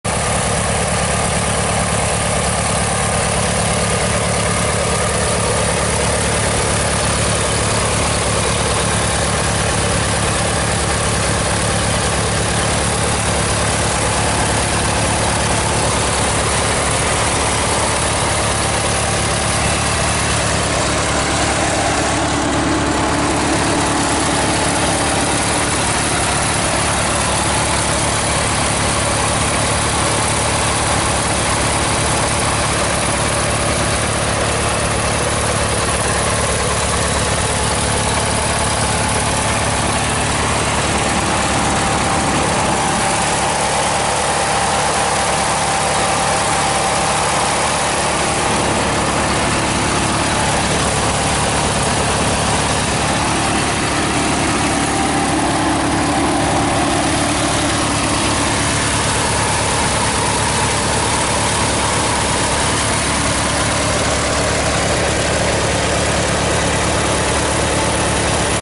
1986 Murray 6-39004 garden tractor.
My Murray 6-39004 garden tractor idling out in the yard.